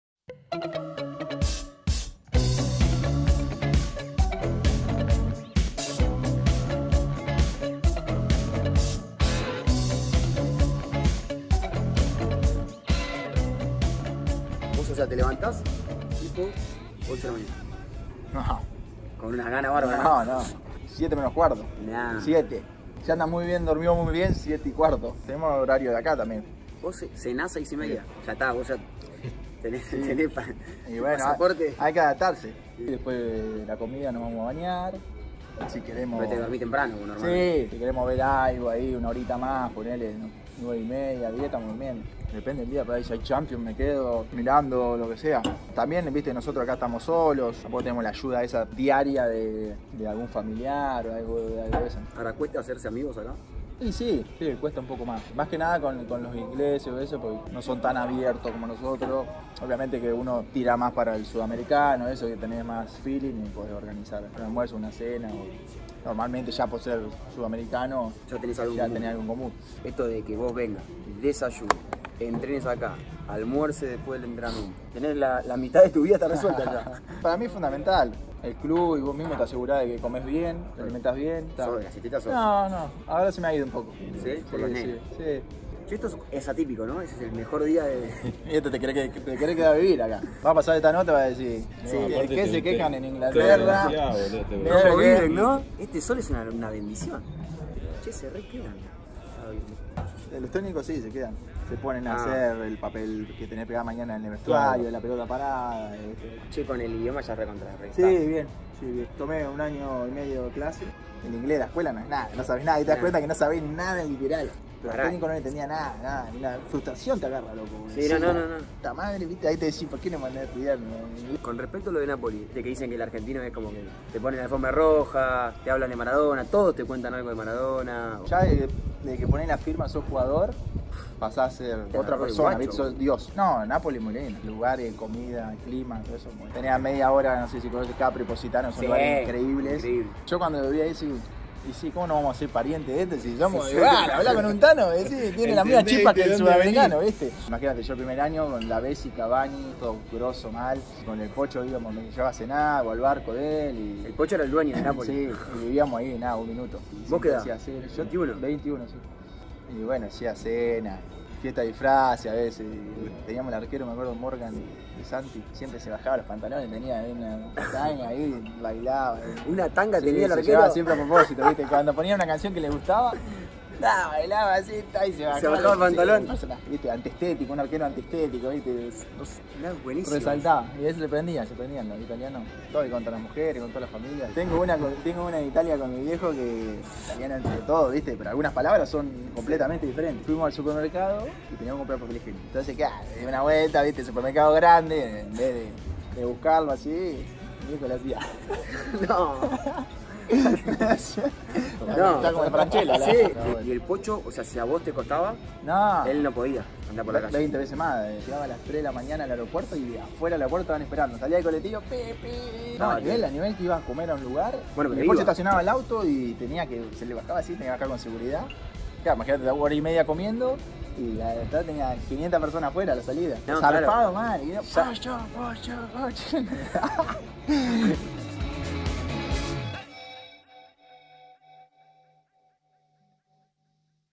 Las anécdotas del Pocho Lavezzi en Napoli!!! Por Fede Fernandez.
El defensor de Newcastle United nos habla de sus epocas de jugador de Napoli, cuando llegó a Italia con solo 21 años e integró, según él, uno de los planteles más divertidos de su carrera! IMPERDIBLE!!!

iSUSCRIBITE!